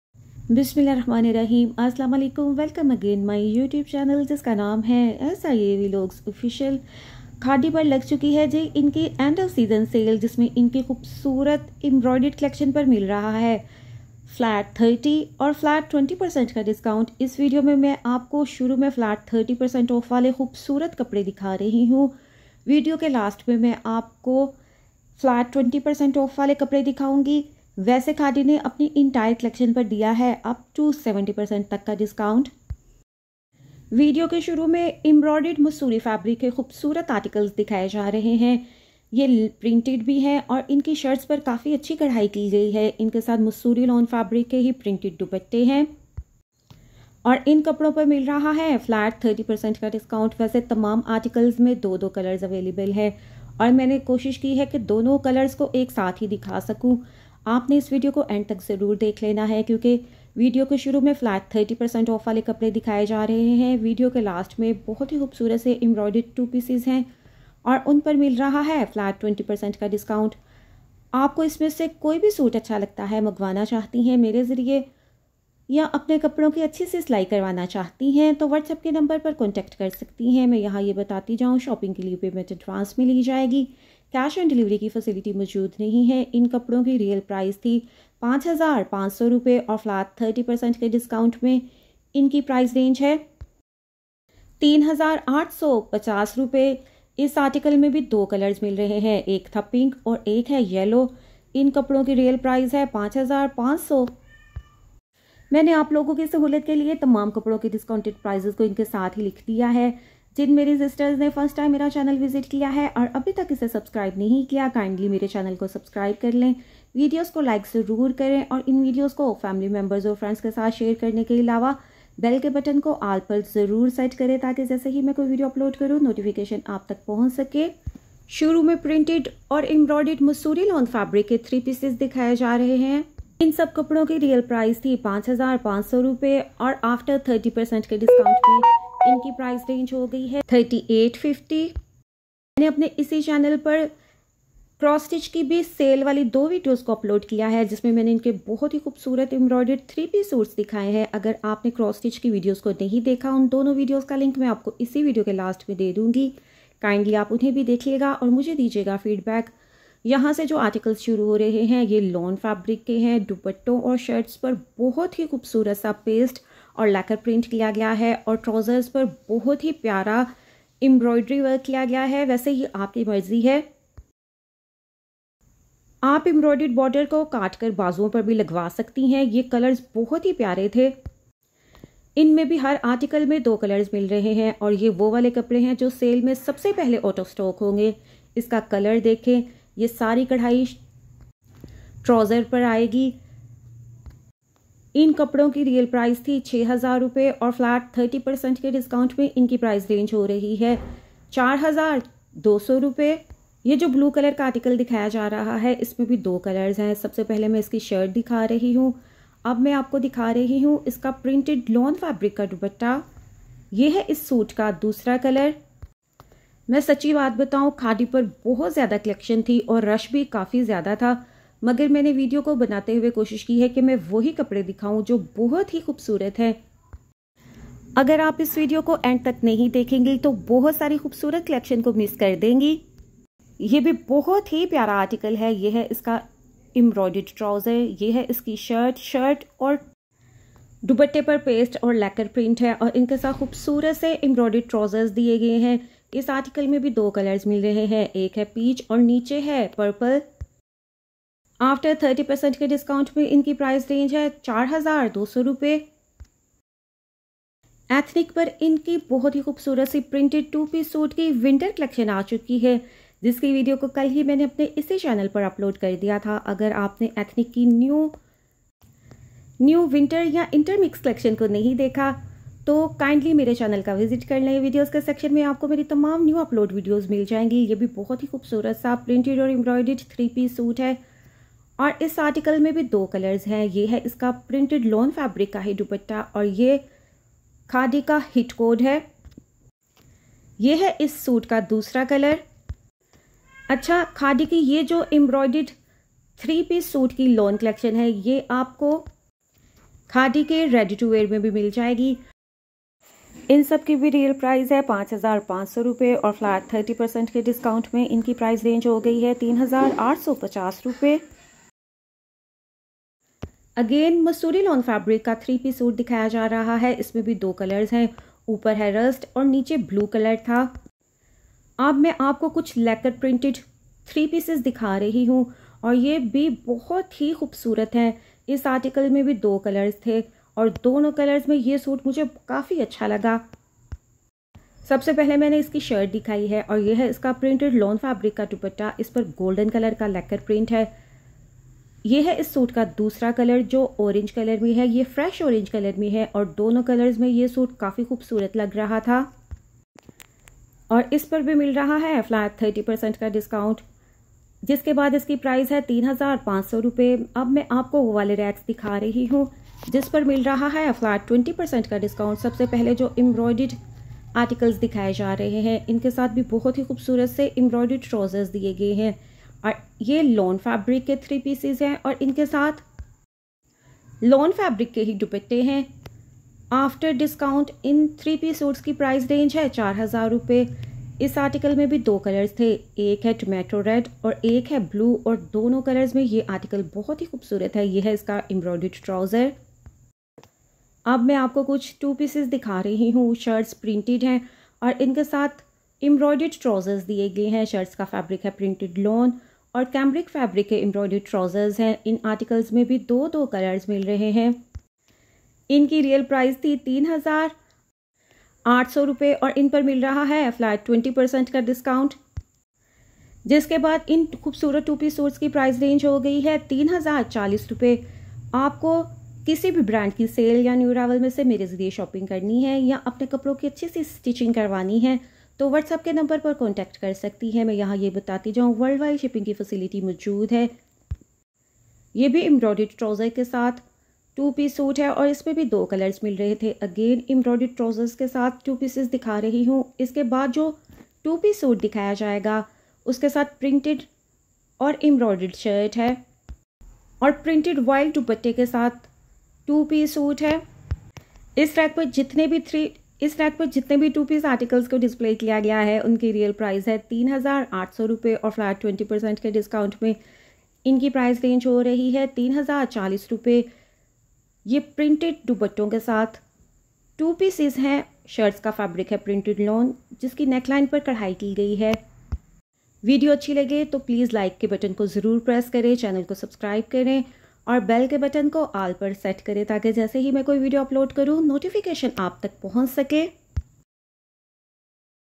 Khaadi sale flat 70% off on entire stock today/Khaadi season end sale 24/SIA Vlogs official
SIA vlogs official aoa.
Khaadi sale on unstitched collection